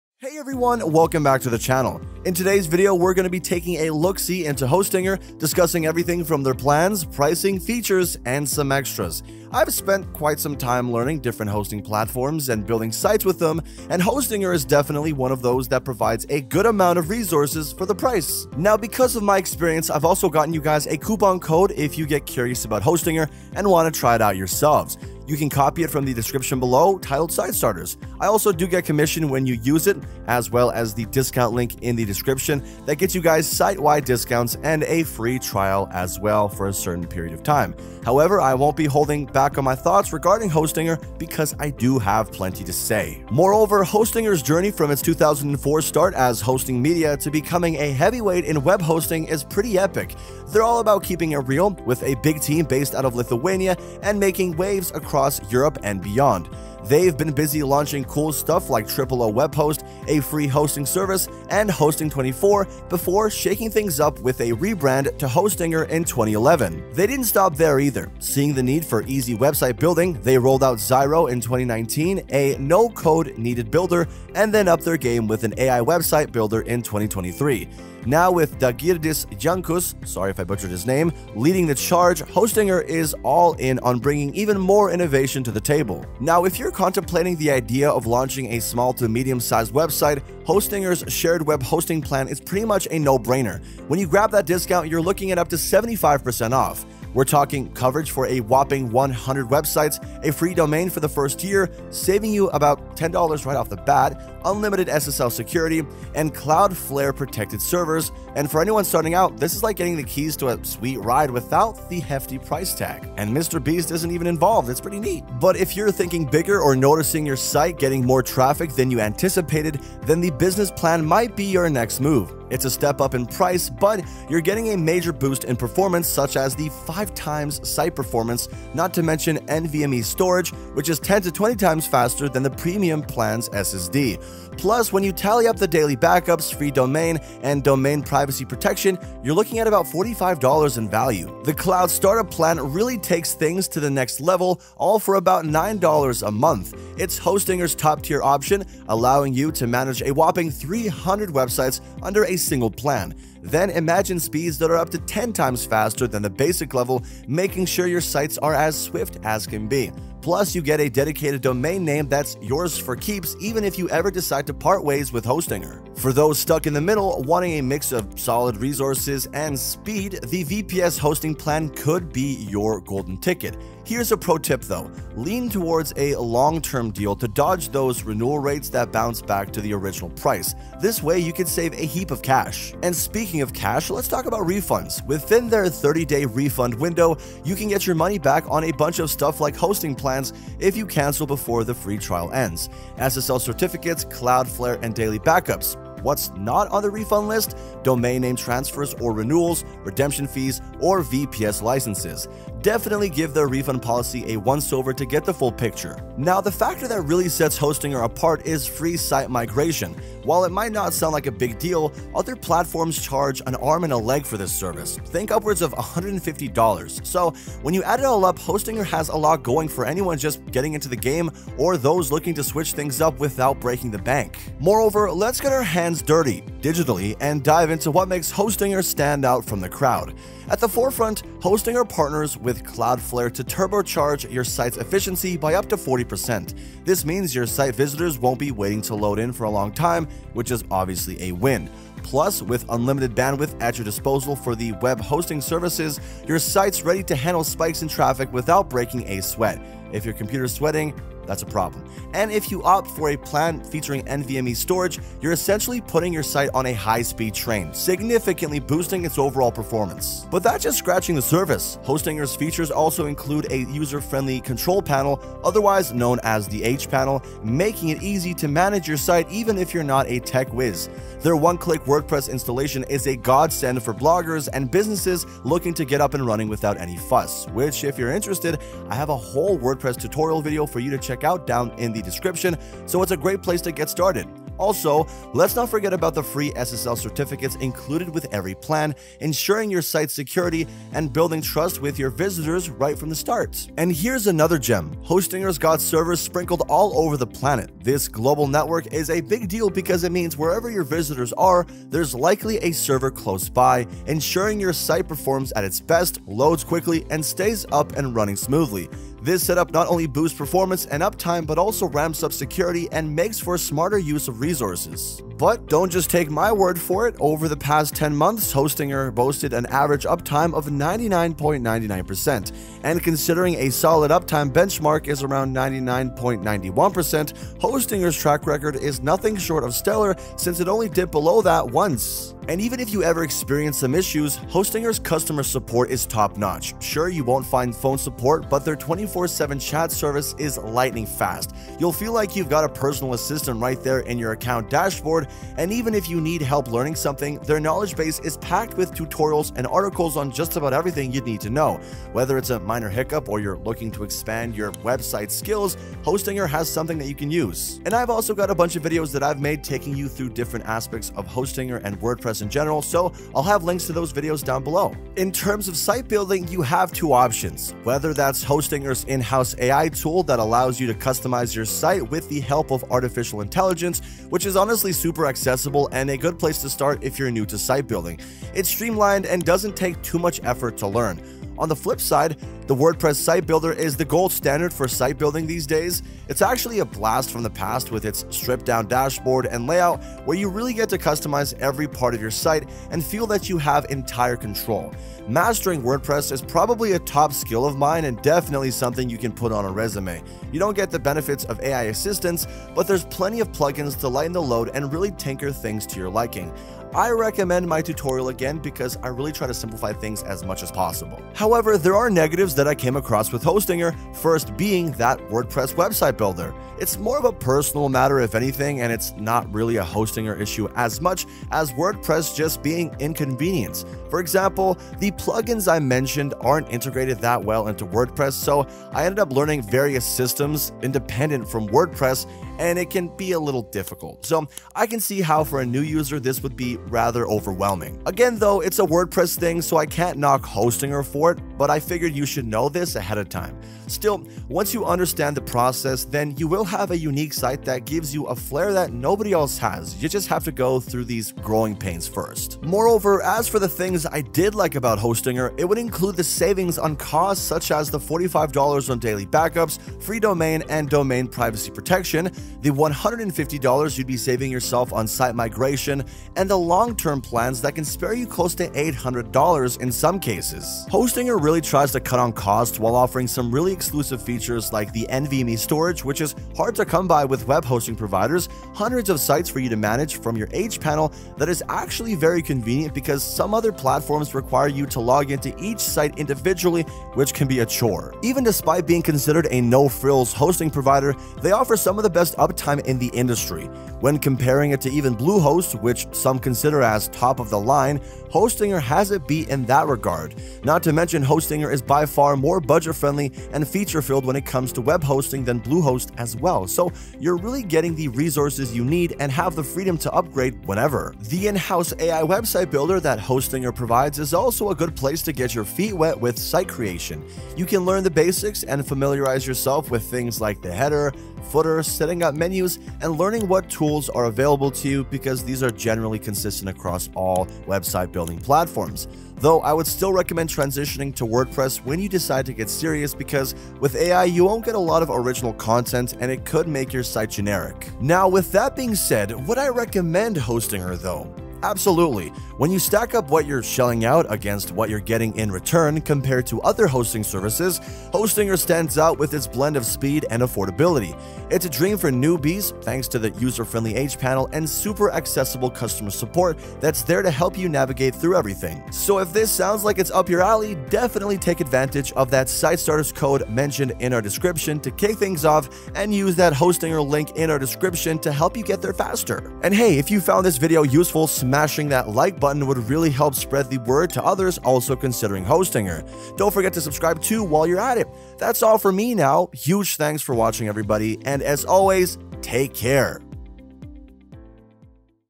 Hostinger Review 2026 : Budget-Friendly, but is it Worthwhile?
Hostinger Review: Budget-Friendly, but is it Worthwhile? Uncover insights for smart hosting decisions.

Timestamp Sections:
00:00 Intro
00:43 History of Hostinger
01:26 Plans and Pricing
03:03 Refund Policy
03:26 Free Website Migration
03:45 Standout Features
05:32 Customer Support
06:06 Site Building
07:36 Cost Saving Features
08:53 Recommendation
09:29 Outro

Welcome back, everyone! Today's video dives into the world of hosting with a focus on Hosting Her. We'll explore their plans, pricing, features, and some additional perks. With personal experience in different hosting platforms, Hosting Her has stood out to me for its balance of resources and affordability. Let's delve into what makes Hosting Her a noteworthy player in the hosting realm.

Hosting Her has come a long way since its inception in 2004 as Hosting Media. Evolving into a heavyweight in web hosting, they've made significant strides, launching services like Triple A Web Host and Hosting 24, before rebranding as Hosting Her in 2011. Their commitment to innovation is evident with the introduction of Zyro in 2019, a no-code builder, and an AI website builder in 2023, led by Dougie Juncus.

For small to medium-sized websites, Hosting Her's shared web hosting plan offers substantial benefits, including up to 75% off, coverage for 100 websites, free domain for the first year, unlimited SSL security, and Cloudflare protected servers. Moving up to the Business Plan brings enhanced performance features like five times site performance, NVMe storage, daily backups, and domain privacy protection.

Meanwhile, the Cloud Startup Plan elevates capabilities with the management of 300 websites, unparalleled speeds, and a dedicated domain name. For those seeking a balance of resources and speed, the VPS hosting plan offers a golden opportunity, especially when opting for long-term deals to dodge renewal rates.

Hosting Her stands out with its array of features geared towards optimizing website performance and user experience. Partnering with Cloudflare turbocharges site efficiency by up to 40%, ensuring swift loading times and uninterrupted browsing for visitors. Unlimited bandwidth and NVMe storage further enhance performance, guaranteeing seamless handling of traffic spikes and a significant boost in overall speed.

The user-friendly H panel simplifies site management, complemented by one-click WordPress installation for hassle-free setup. Free SSL certificates add an extra layer of security, instilling trust and credibility from the get-go. Hosting Her's global network of servers ensures optimal performance and uptime, boasting an impressive track record of 99.99% uptime over the past 10 months. Their responsive 24/7 chat support and extensive knowledge base offer invaluable assistance to users, whether troubleshooting minor issues or expanding website skills.

While Hosting Her's in-house AI website builder provides a beginner-friendly option, transitioning to WordPress unlocks unparalleled customization and control, albeit with a slight learning curve. Despite minor inconveniences with WordPress integration, Hosting Her's cost savings, robust features, and stellar performance make it a highly recommended choice for individuals and businesses alike.

Hope you enjoyed my Hostinger Review 2026: Budget-Friendly, but is it Worthwhile? Video.